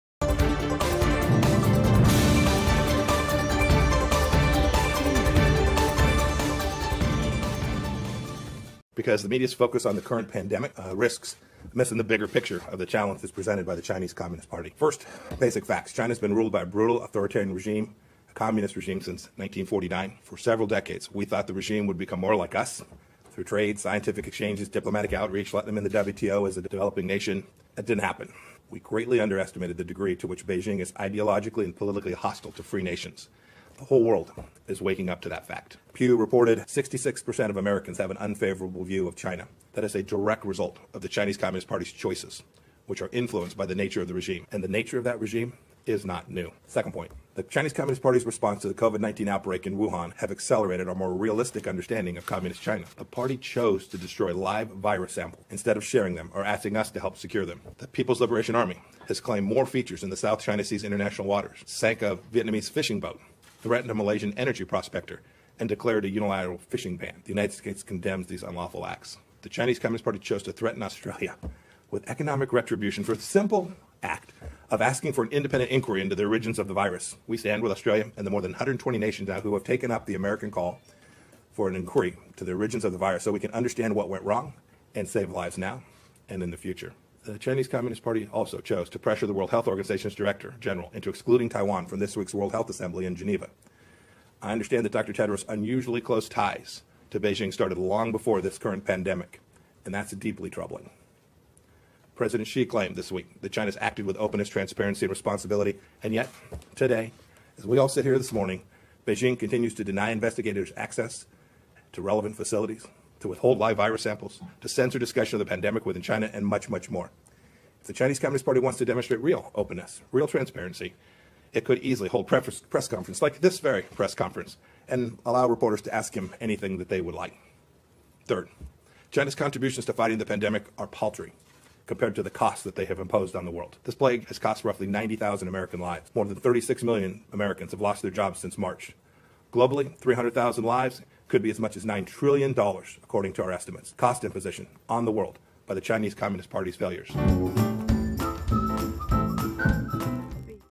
超级重磅「美国正式定性」中共为残暴的独裁专制政权 /蓬佩奥新闻发布会说：1949年以来，中国被残暴专制的共产政权统治，现在美国和全世界已经彻底警醒完全看清了中共真面目。
蓬佩奥新闻发布会说：1949年以来，中国被残暴专制的共产政权统治
现在美国和全世界已经彻底警醒，完全看清了中共真面目。
他还历数中共最近的种种恶行，120国将联合调查病毒起源。
整个讲话只针对中共，不针对中国人民。